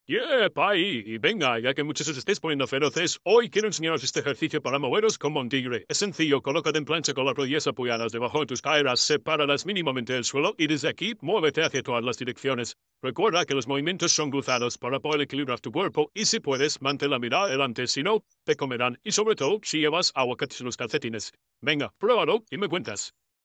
Muévete como un tigre 🐅
MÁS INFO ℹ️

Este es un ejercicio de fuerza, estabilidad y coordinación, podríamos llamar un 3x1.

Más allá de tratar más peso en Sentadilla o Press de Banca, busca cada vez moverte mejor y llegar a más lugares con tu cuerpo.

Un abrazo 🐅